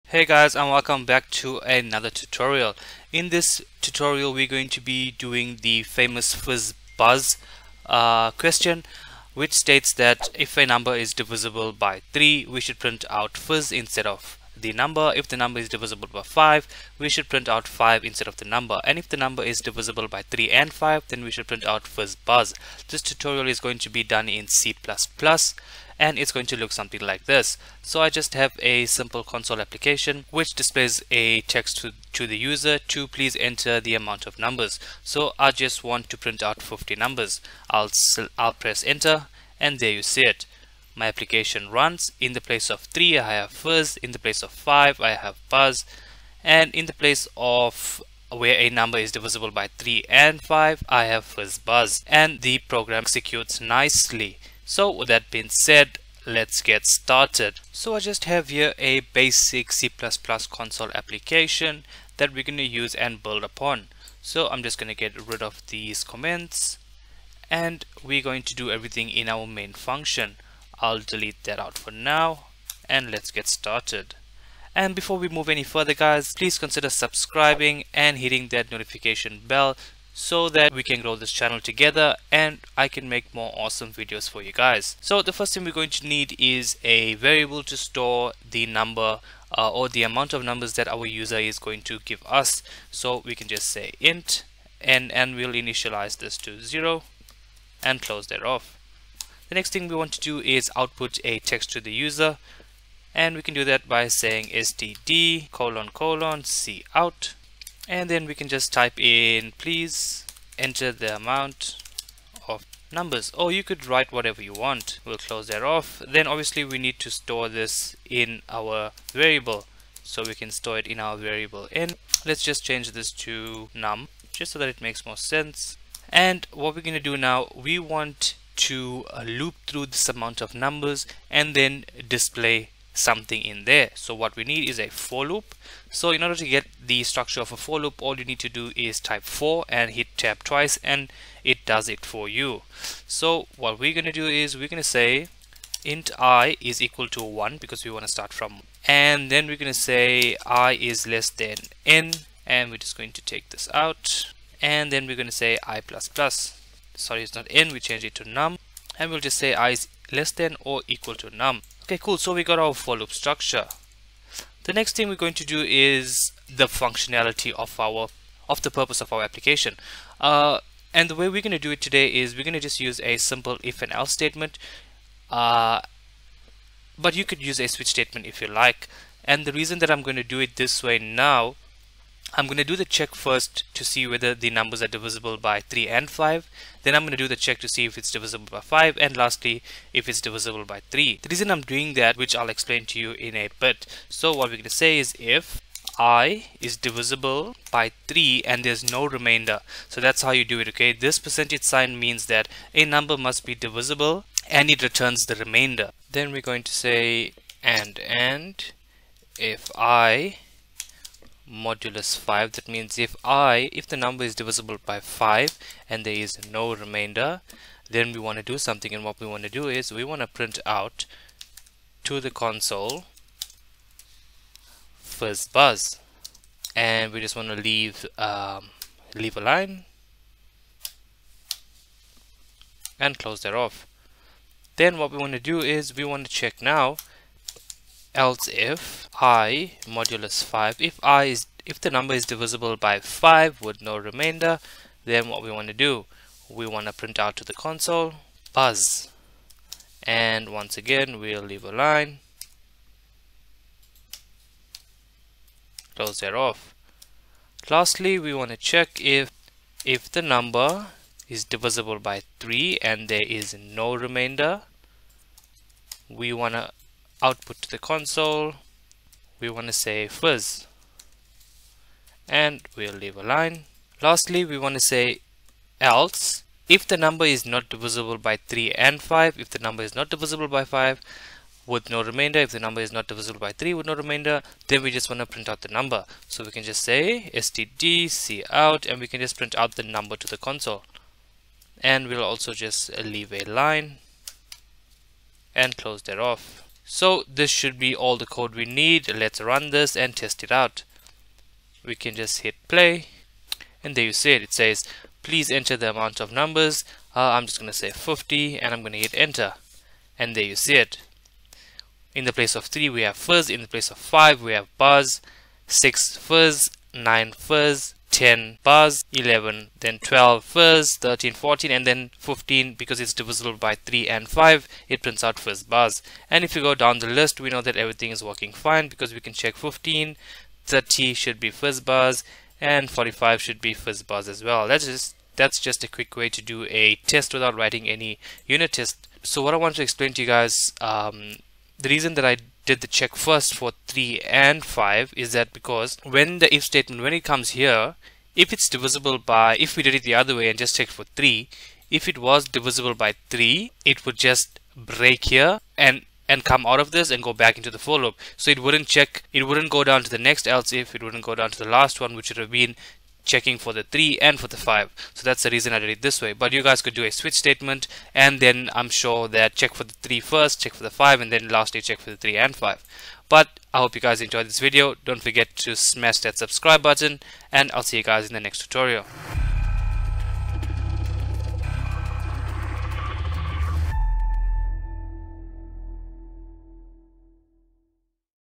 Fizz Buzz SOLVED - C++ Tutorial 2021
Learn how to solve the famous FIZZ BUZZ problem with a basic C++ console application.